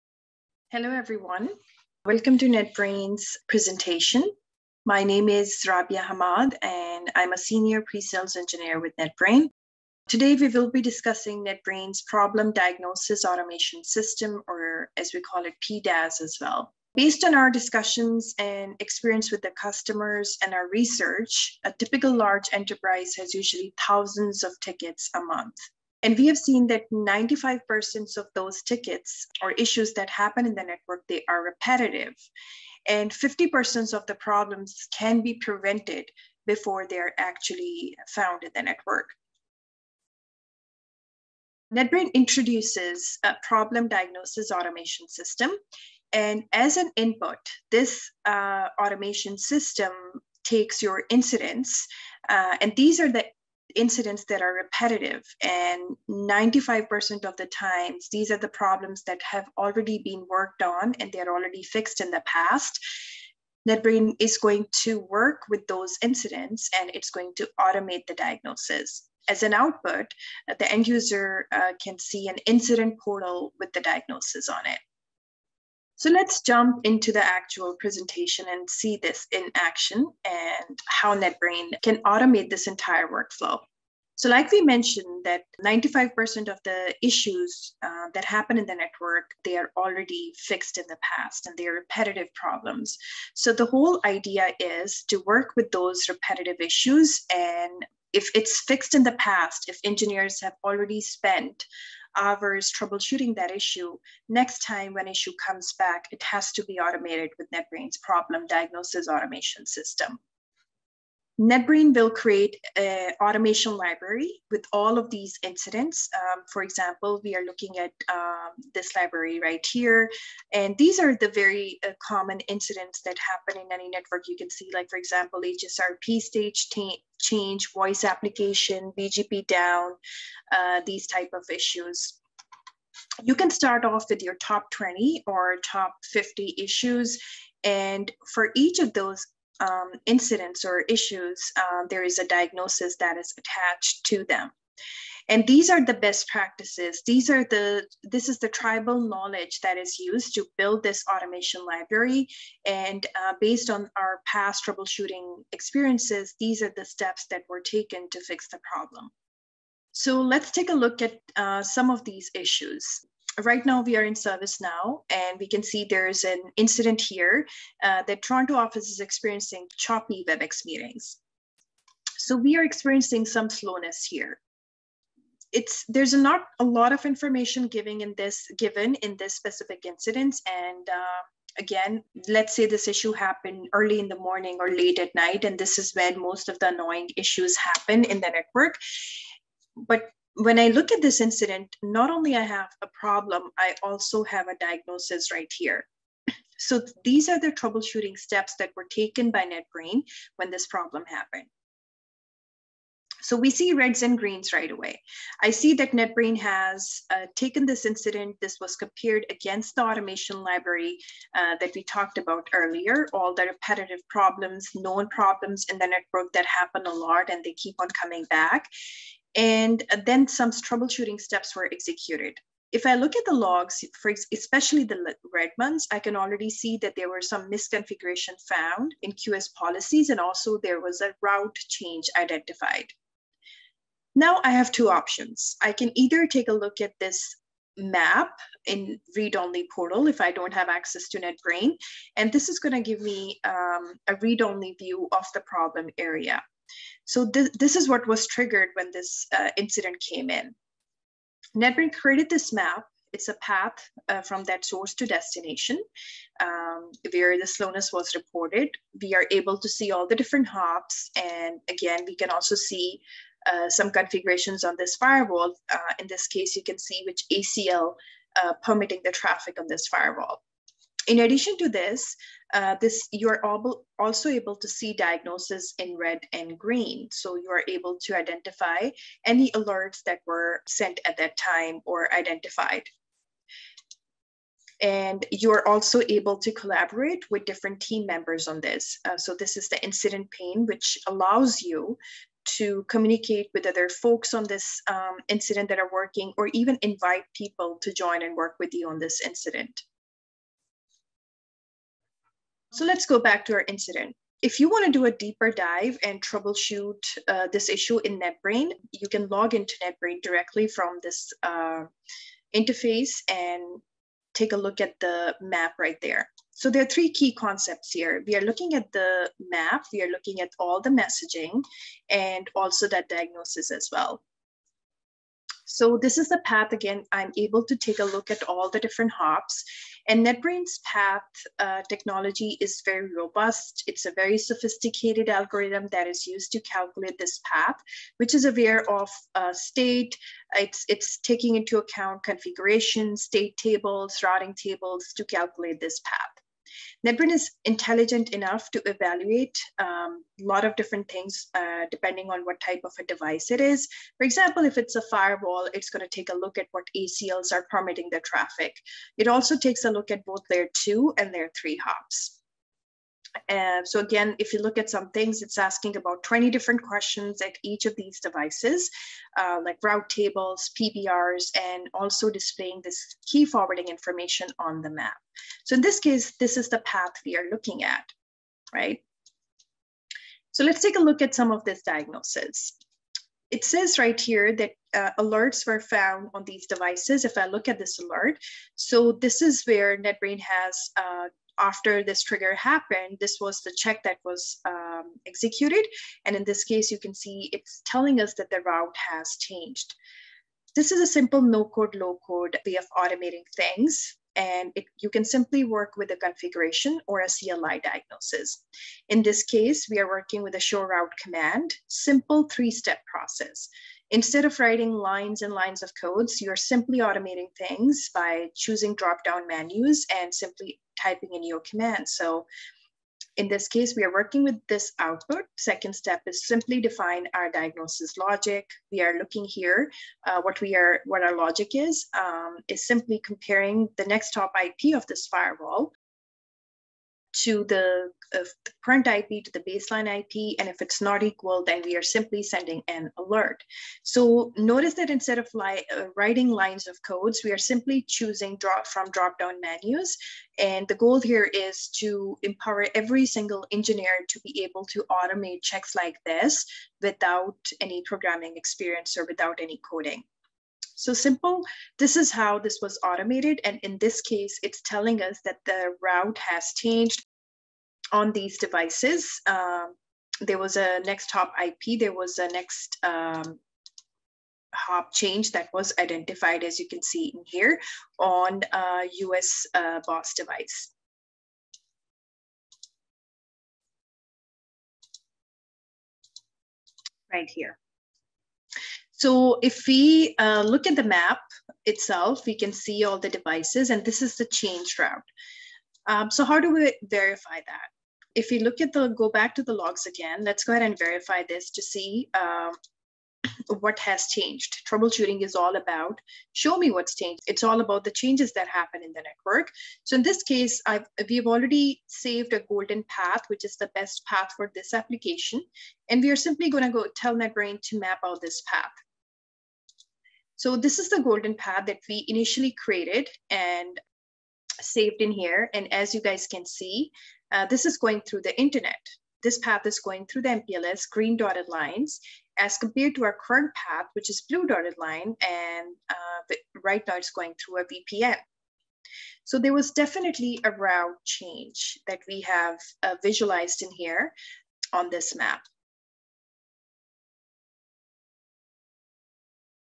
NetBrain's Problem Diagnosis Automation System (PDAS) in 10 mins
Learn the concept behind NetBrain's Problem Diagnosis Automation System and see it in action - in under 10 mins!

Why NetBrain PDAS?
👉 NetOps spends 2/3 of their time on PROBLEM DIAGNOSIS alone, and although a typical large enterprise has 1000's of tickets each month, 95% of them are repetitive. In this video, you will see Rabia Hammad, Senior Pre-Sales Engineer, demonstrate how NetBrain PDAS automates the diagnosis of network problems. Instead of manually troubleshooting each network problem from scratch, you can troubleshoot manually once and then automate the diagnosis. So, the next time a 'similar' problem occurs, NetBrain can diagnose it automatically before a human ever gets involved.